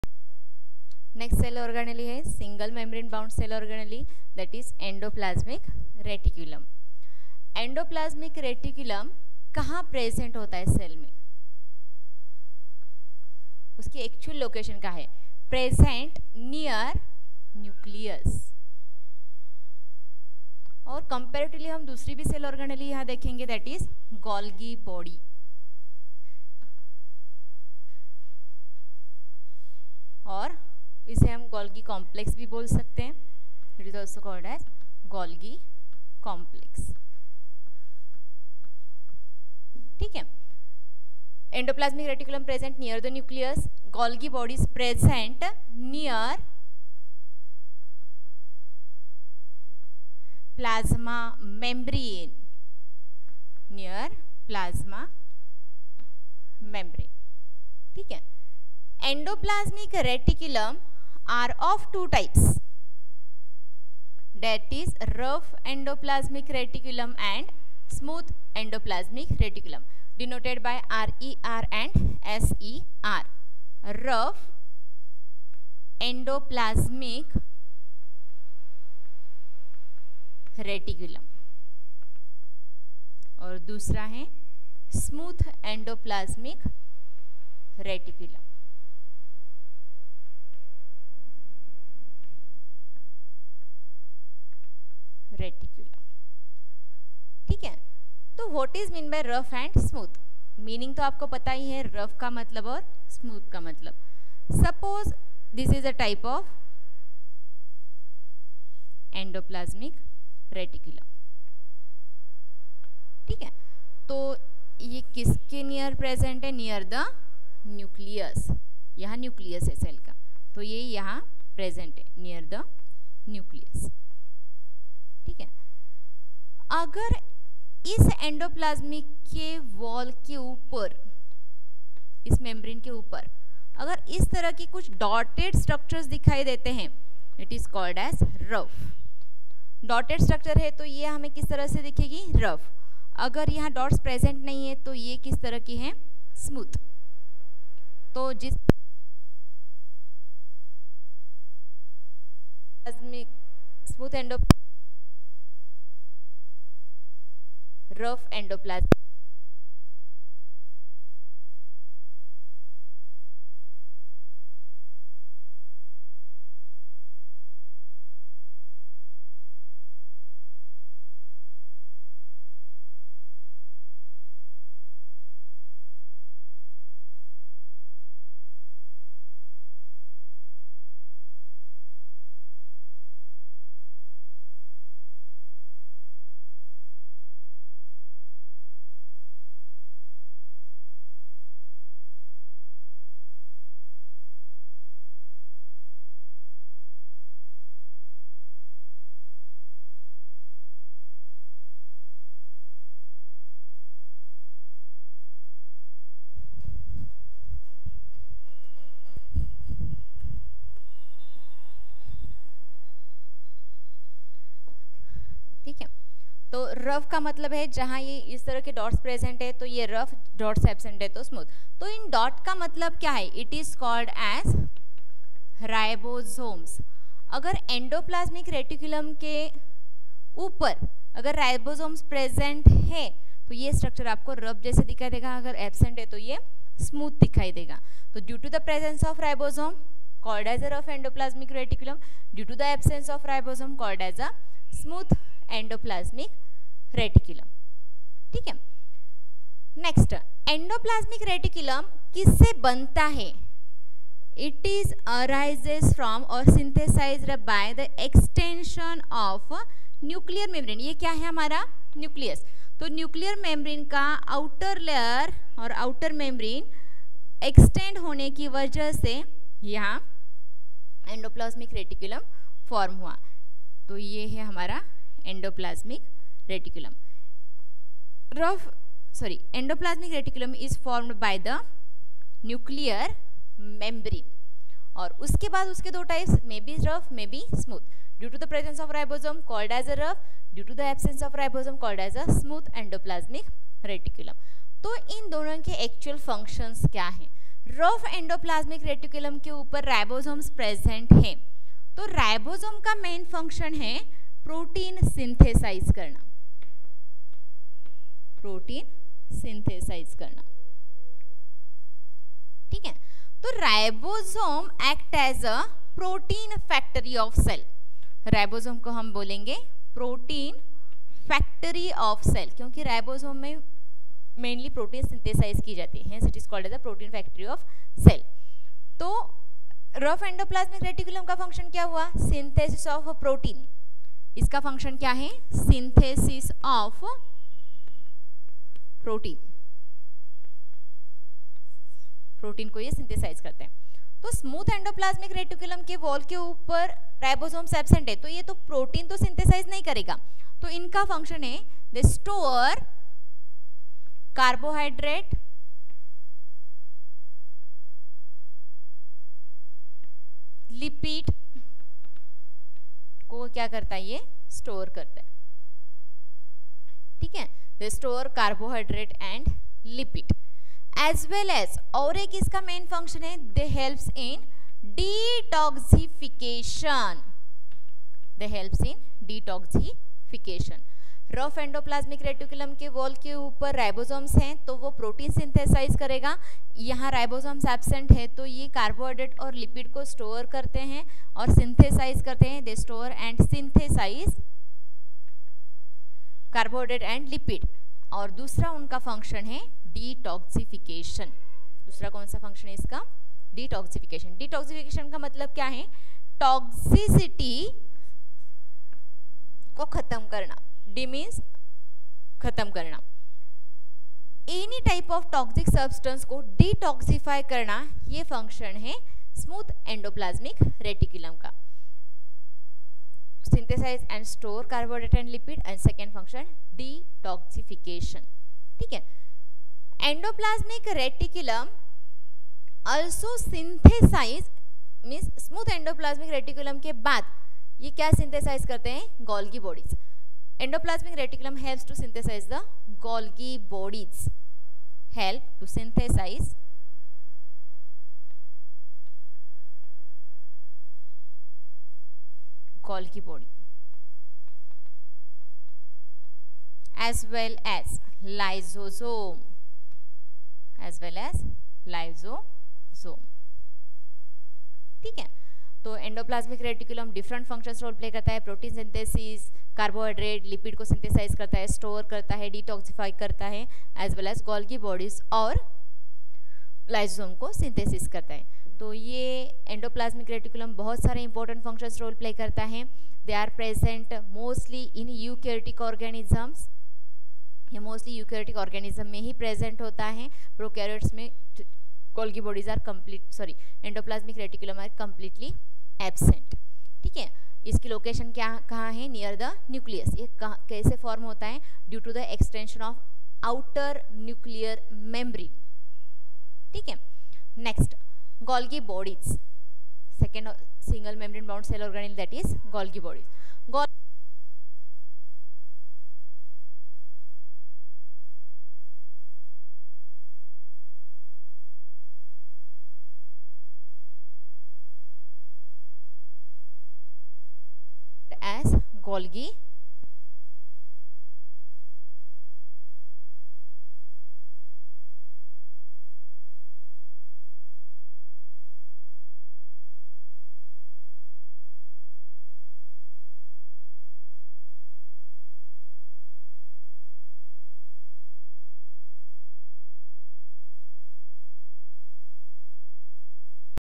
lec 6 class 9 The fundamental unit of life Endoplasmic reticulum
Class 9 : The Fundamental Unit Of Life - Theory Video Lecture + NCERT Exercise + Online Test + Test Solution.